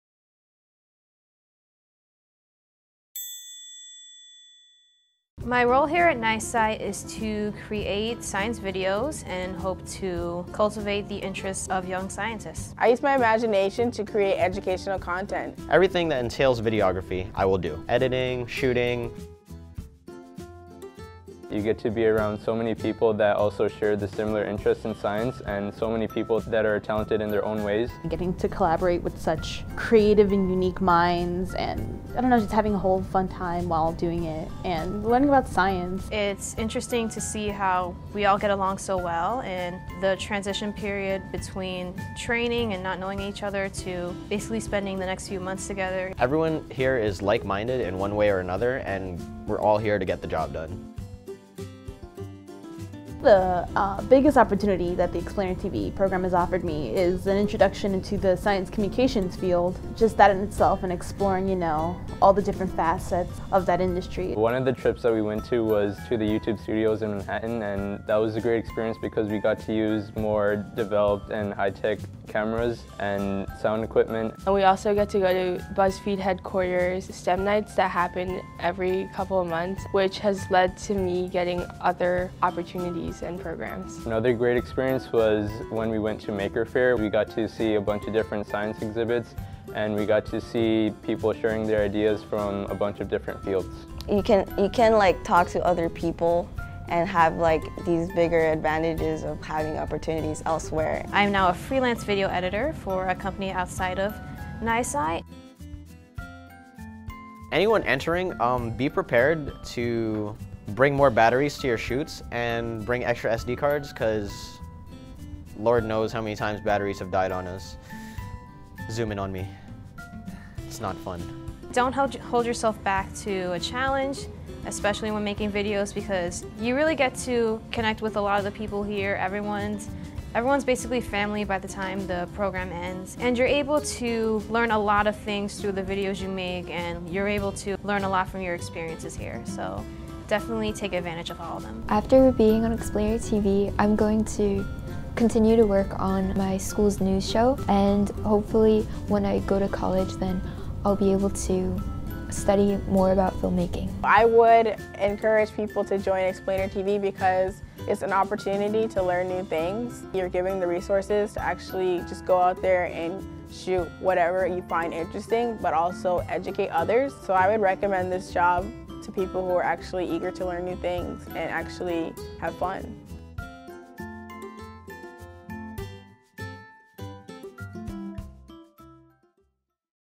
Explainer TV: NYSCI's Youth Science Communication Program
An SCL Institute initiative, Explainer TV engages Explainers in learning digital media production that highlights the Explainers, science, and NYSCI. Explainers become video producers and science, technology, engineering, and math (STEM) advocates as they learn video production skills. They create short videos, beginning with storyboarding, writing, interviewing, lighting, and shooting, to final video and audio post-production. Explainers learn to develop and communicate their ideas – frequently highlighting the importance of STEM in their lives and for future generations.

Producer: Jairo Cuenca
Director: Jairo Cuenca
Interviewers: Rachel, Genesi, Jairo Cuenca
Camera: Ryan
Audio: Genesi
Lighting: Byron
Editor: Kevin Cunningham